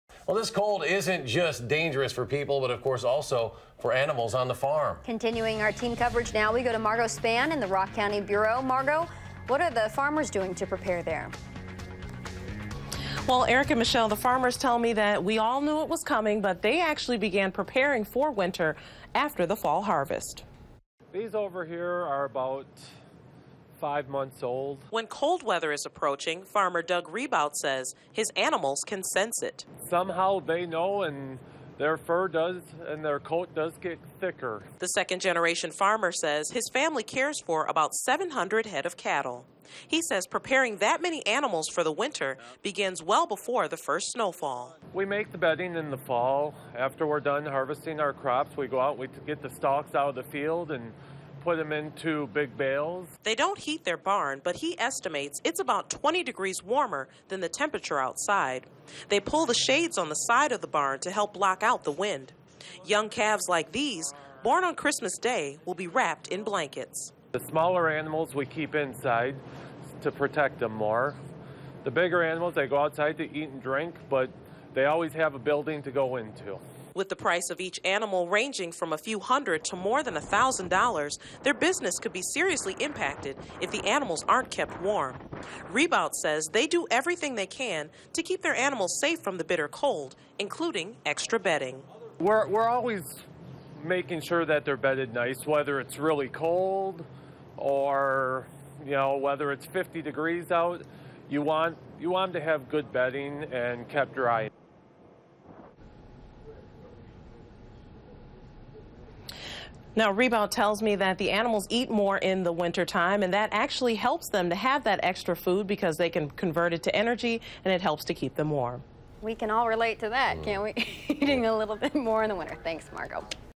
Farmers buckle down to protect animals from harsh weather
The chilly winds and freezing temperatures can be difficult for animals
living outside. When cold weather is approaching, Janesville farmer
Doug Rebout says his animals can sense it.

Margo Spann reports.